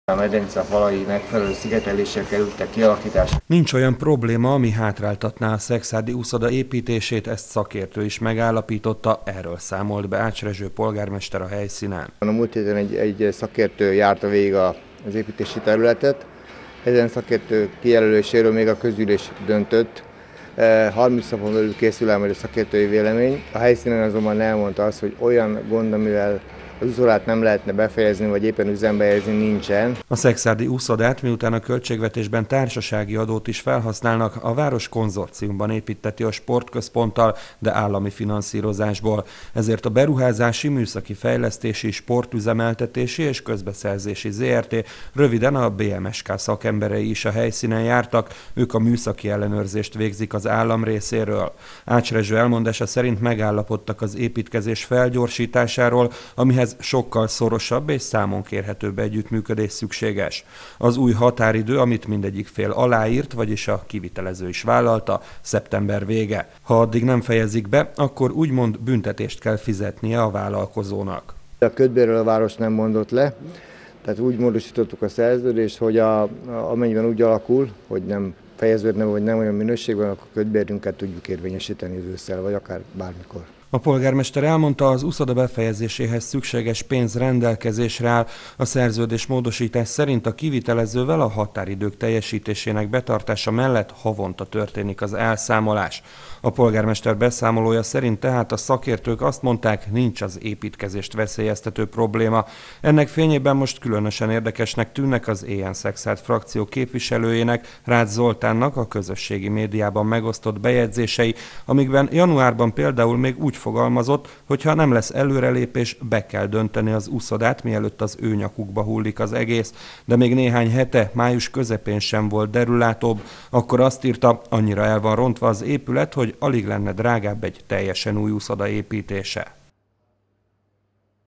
Szeptemberre elkészülhet a szekszárdi uszoda - a szakértők szerint nem kell leállni a beruházással
Mehet tovább az építkezés, szeptemberre elkészülhet az új szekszárdi uszoda. A szerződésmódosítást már aláírták és a műszaki szakemberek sem látnak olyan problémát, ami miatt le kellene állni a beruházással. Erről tartott sajtótájékoztatót Ács Rezső polgármester.